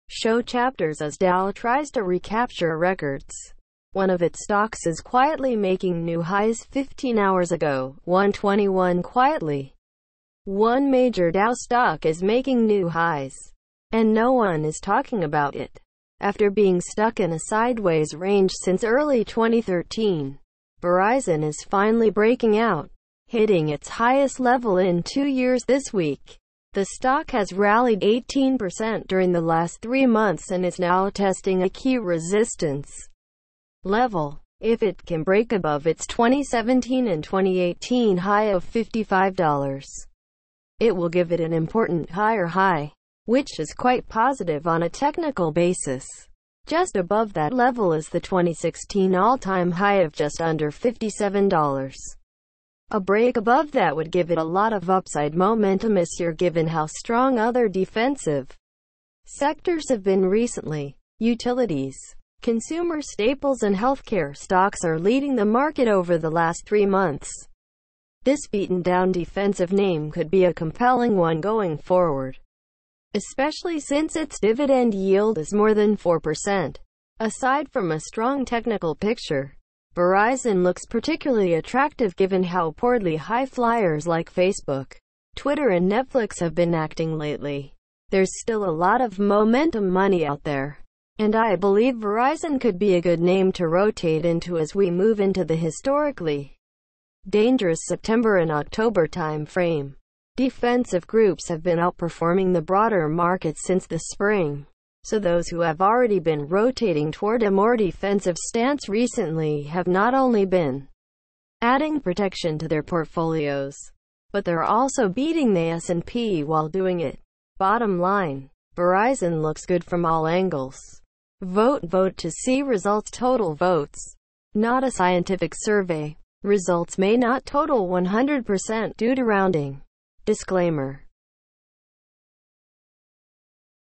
One Dow stock is quietly making new highs for the first time in years
show chapters                                       As Dow tries to recapture records, one of its stocks is quietly making new highs                              15 Hours Ago |  01:21 Quietly, one major Dow stock is making new highs, and no one is talking about it. After being stuck in a sideways range since early 2013,  Verizon  is finally breaking out, hitting its highest level in two years this week. The stock has rallied 18 percent during the last three months and is now testing a key resistance level. If it can break above its 2017 and 2018 high of $55, it will give it an important higher high, which is quite positive on a technical basis. Just above that level is the 2016 all-time high of just under $57. A break above that would give it a lot of upside momentum. Given how strong other defensive sectors have been recently  utilities, consumer staples and health care stocks are leading the market over the last three months  this beaten down defensive name could be a compelling one going forward. Especially since its dividend yield is more than 4 percent. Aside from a strong technical picture, Verizon looks particularly attractive given how poorly high flyers like  Facebook ,  Twitter  and  Netflix  have been acting lately. Theres still a lot of momentum money out there, and I believe Verizon could be a good name to rotate into as we move into the historically dangerous September and October time frame. Defensive groups have been outperforming the broader market since the spring, so those who have already been rotating toward a more defensive stance recently have not only been adding protection to their portfolios, but theyre also beating they  S&P  while doing it. Bottom line: Verizon looks good from all angles. Vote Vote to see results Total Votes:  Not a Scientific Survey. Results may not total 100% due to rounding.